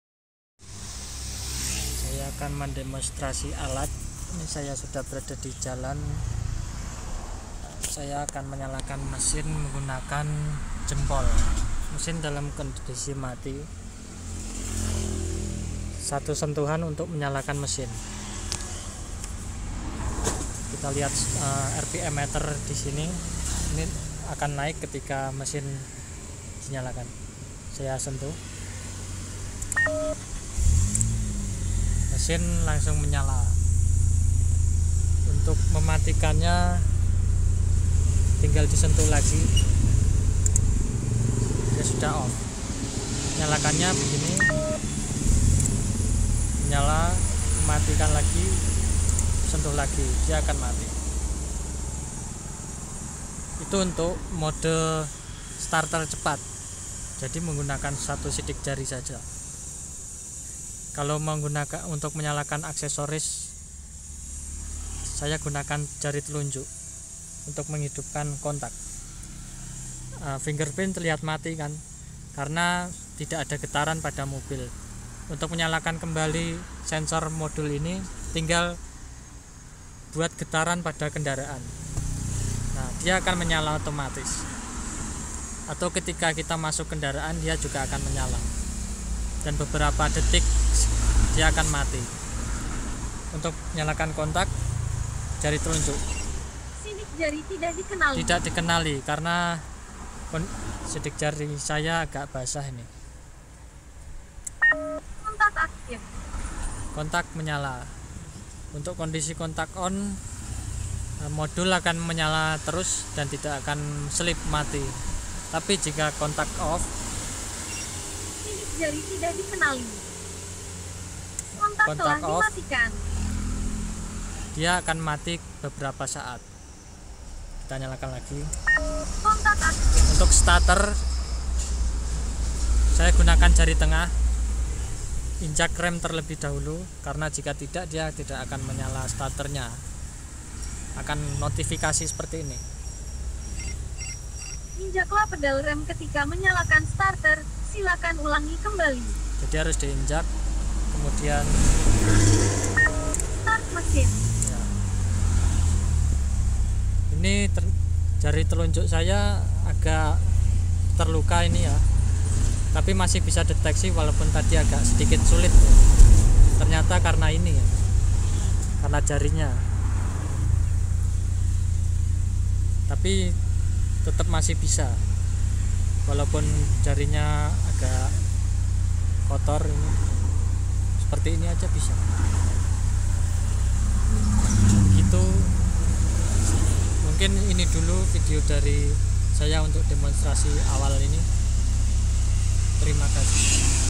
Fingerprint Car Starter with Voice Speech, starter mobil sidik jari, Biometric Security
Demo awal
Biometric Security Fingerprint Car Starter with Voice Notification
Made in Indonesia (saya sendiri)